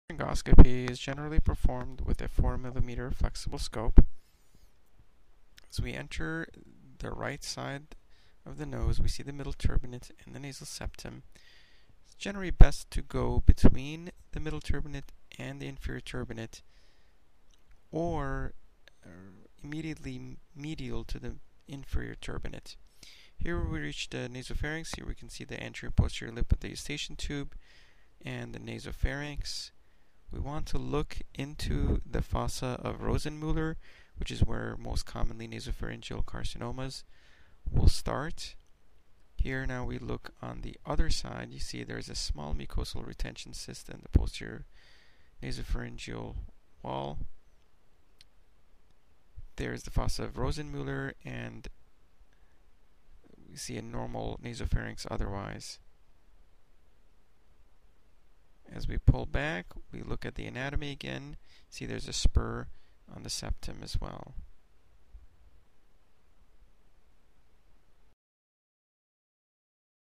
nasopharyngoscopy (looking in the back of the nose)
This video shows a nasopharyngoscopy. The nasopharynx is the area of the back of the nose where the back of the nose opens to the pharynx. The opening of the Eustachian tubes is also in that area.